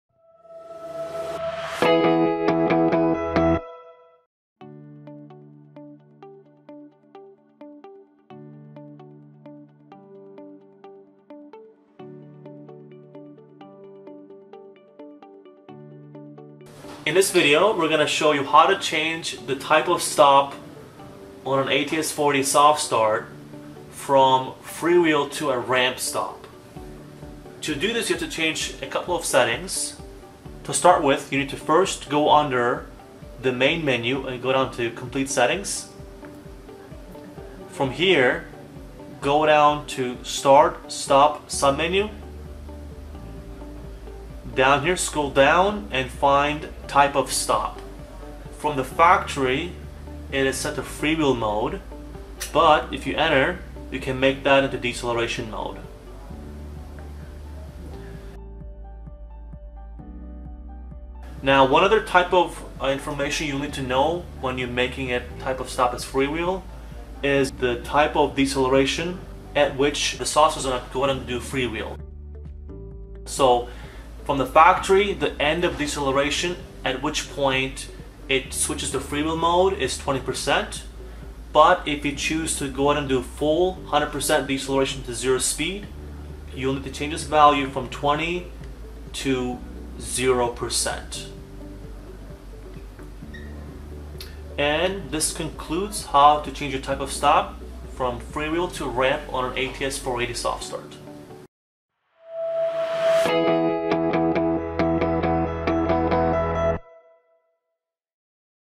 How to set an ATS480 to ramp down instead of freewheel stop? | Schneider Electric
This video explains how to set a ramp down instead of a freewheel stop on an ATS480 Soft Starter.